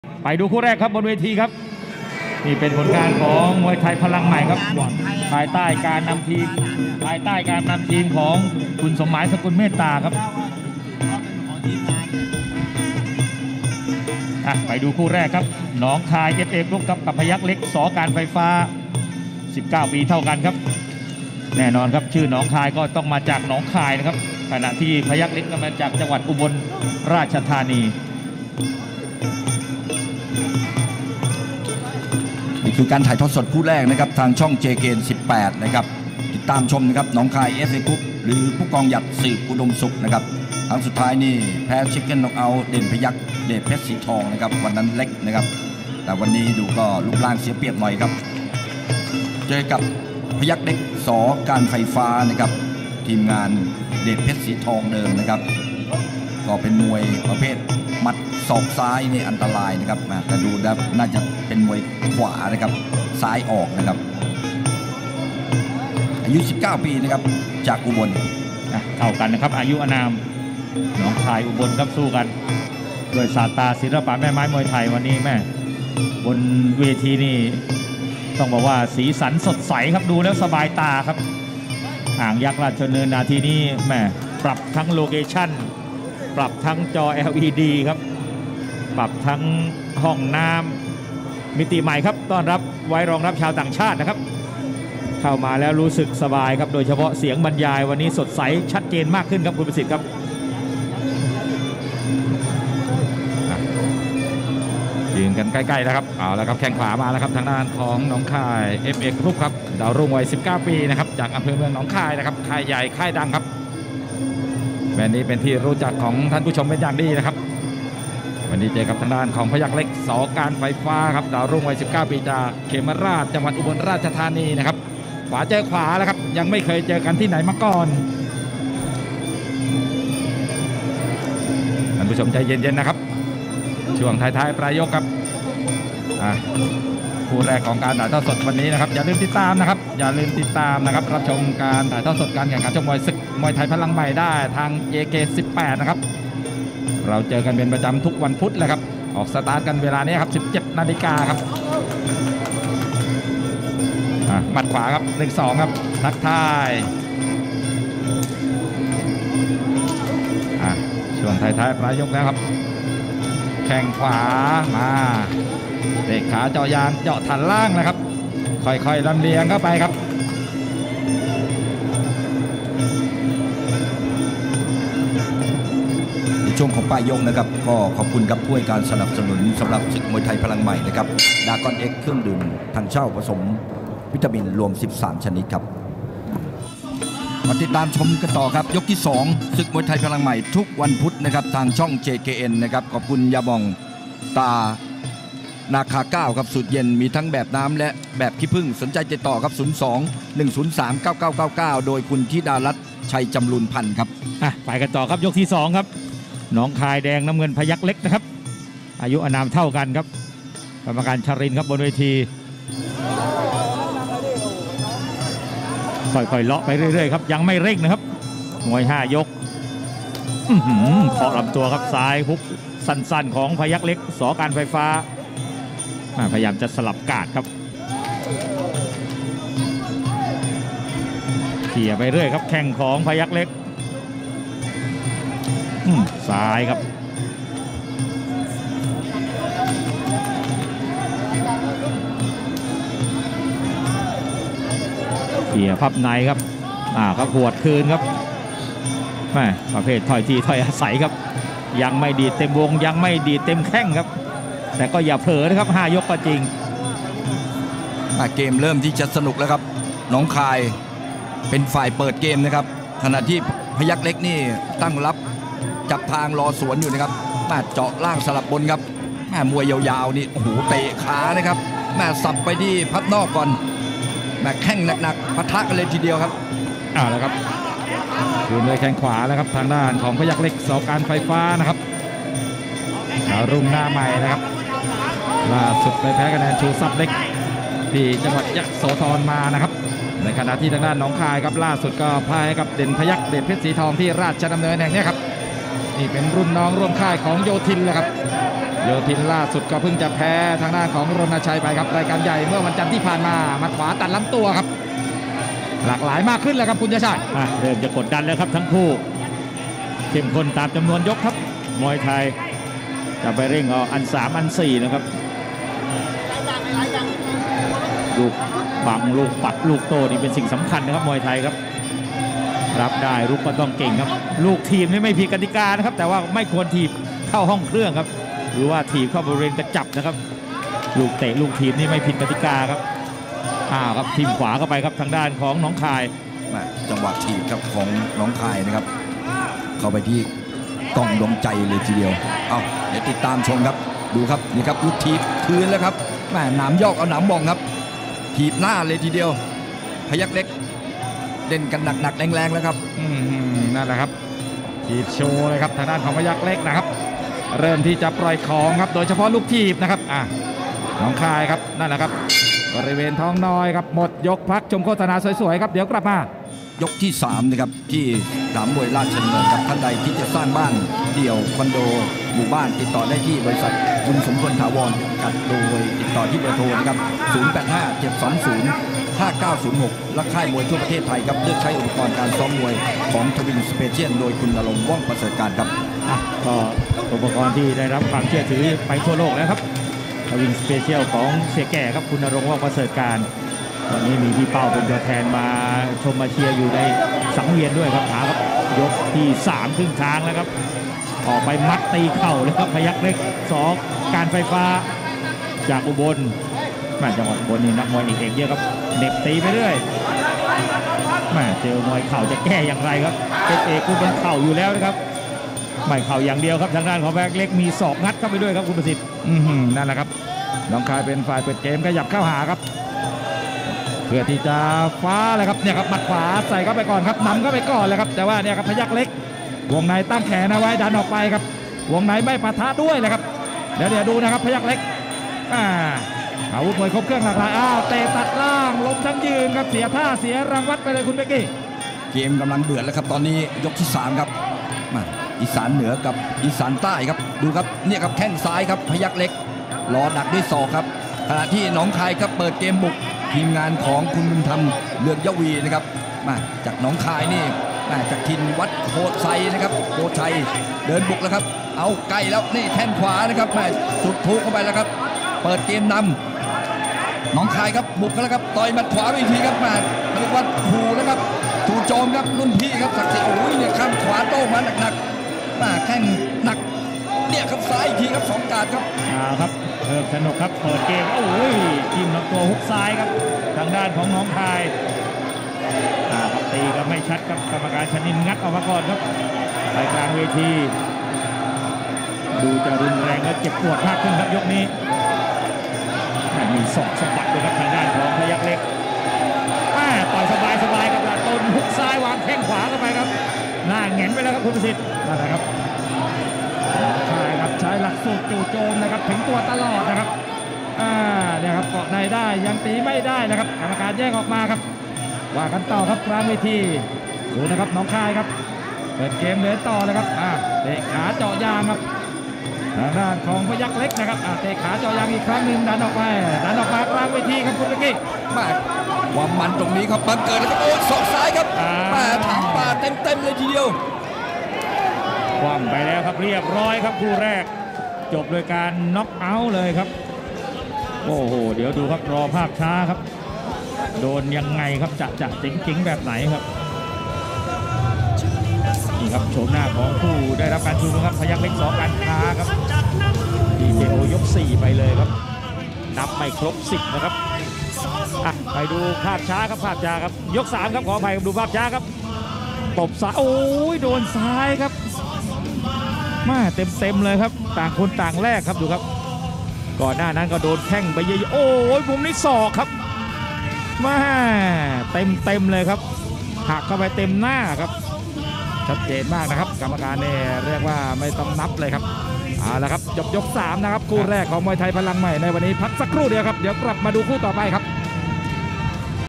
パヤックレック VS ノーンカーイ 2022.6.22「Palangmai Rajadamnern Stadium」【ムエタイ】
[60kg] Payaklek Sor.Karnfaifah (19/175cm) vs NongKhai F.A Group (19/172cm) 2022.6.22 Full Fight 3分5R